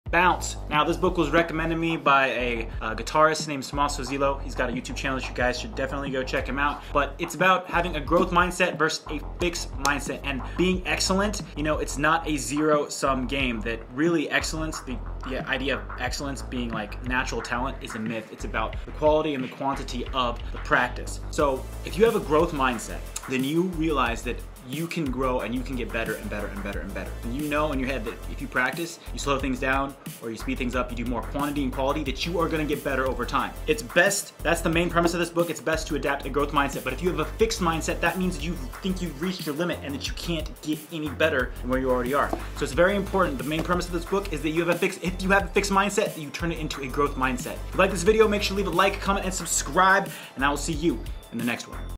Bounce - Book Review/Summary - Growth Mindset
"Bounce" by Matthew Syed unravels the science of success and resilience.

Explore the secrets behind high achievement and learn how to adopt a growth mindset that can help you overcome challenges and reach your goals.